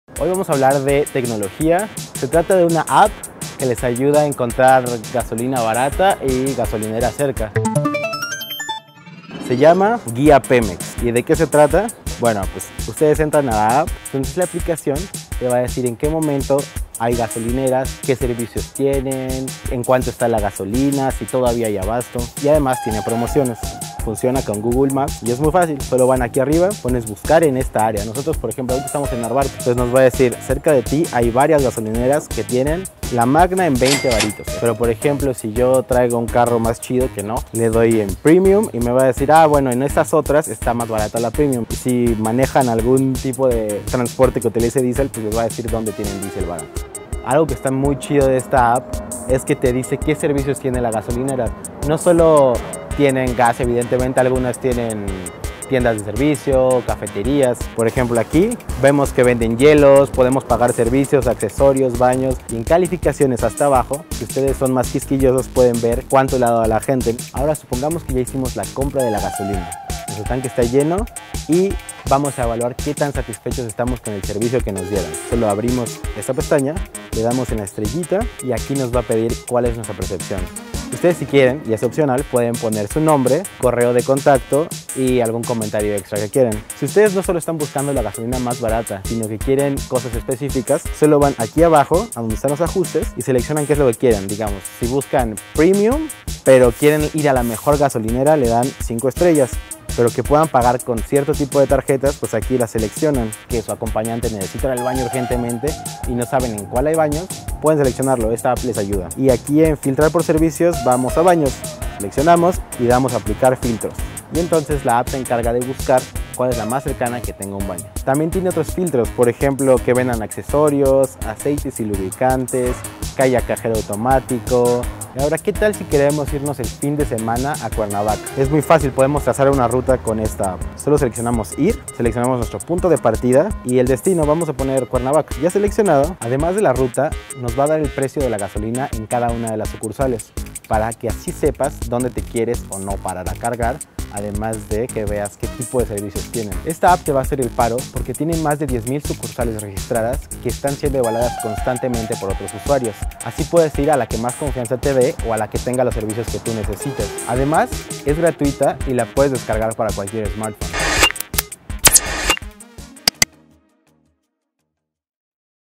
Guía Pemex - Chilango
Sácale provecho a esta app y encuentra gasolina barata en la CDMX